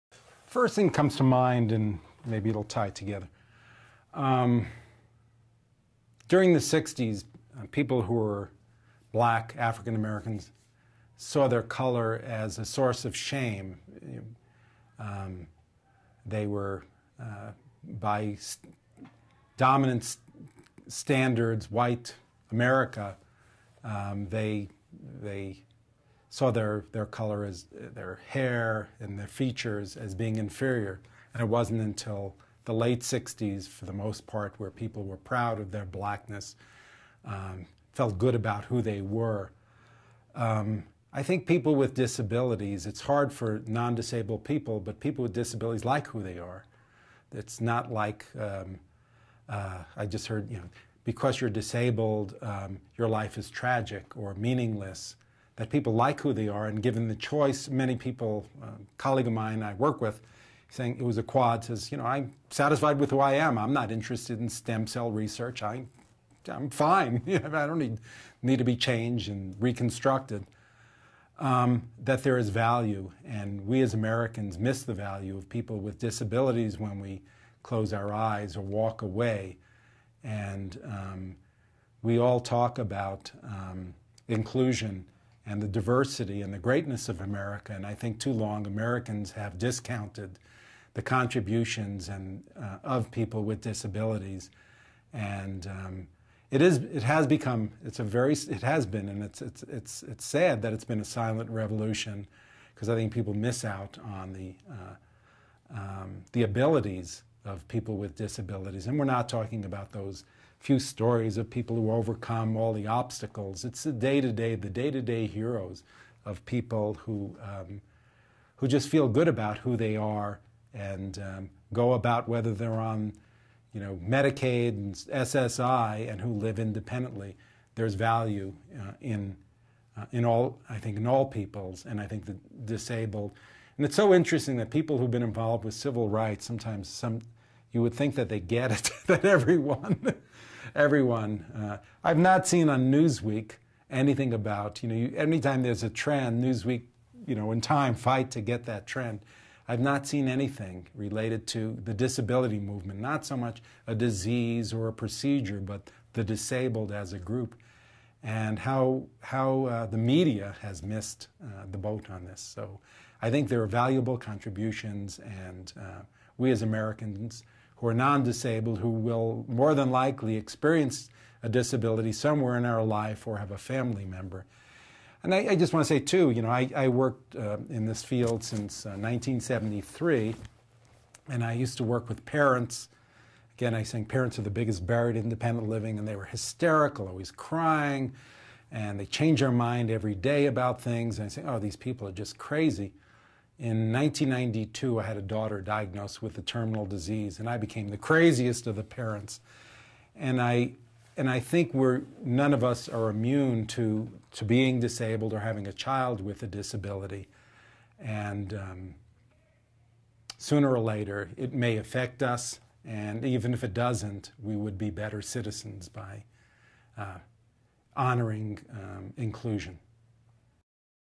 Barry Rosenberg, Denver, CO: "Why This Project is Important"
Barry Rosenberg of Denver, CO answers the question, "Why is the It's Our Story project so important?"

This is one of more than 1,000 interviews that "It's Our Story" has collected in an effort to free the voices of the disability community.